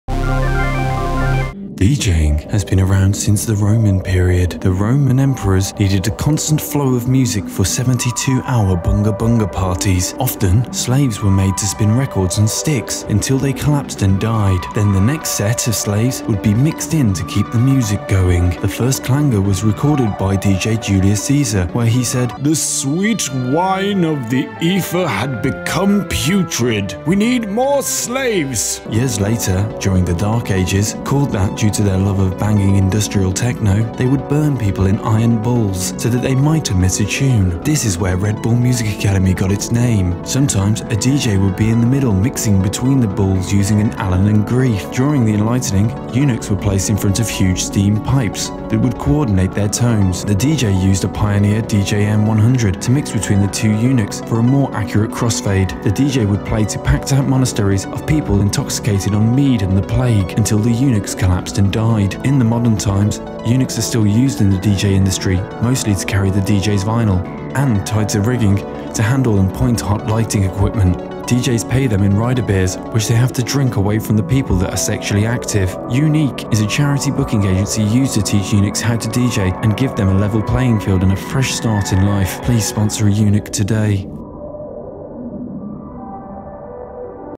A Brief History of DJing
DJ's have been around since time immemorial. Ancient Romans used to need a constant flow of music for giant Bunga Bunga parties and more!